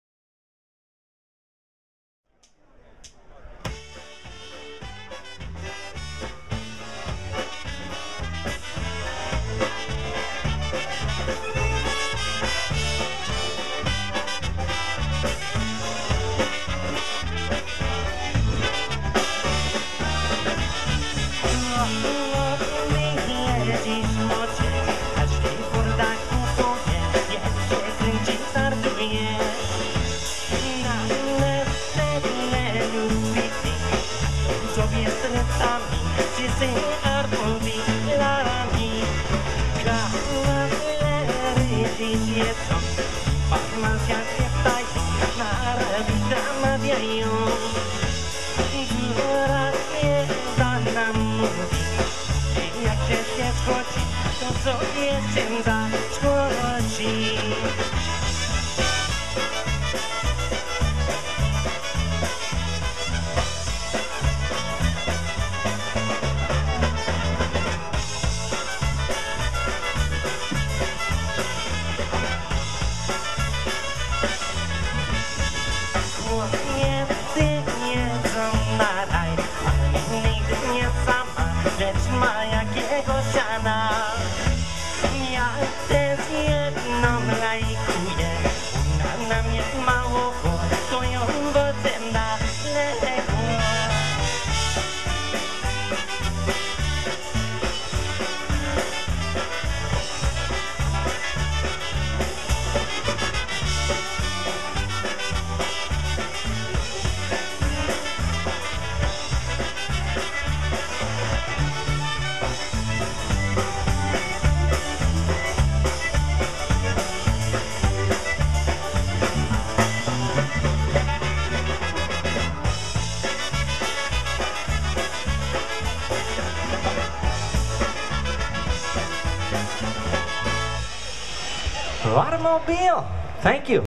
Eddie Blazonczyk and the Versatones
This video features Eddie Blazonchyk and the Versatones (from Chicago)  performing at the Our Lady of Mount Carmel Parish Polish festival in Wyandotte, MI. Click on the above link for more polka and Polish music.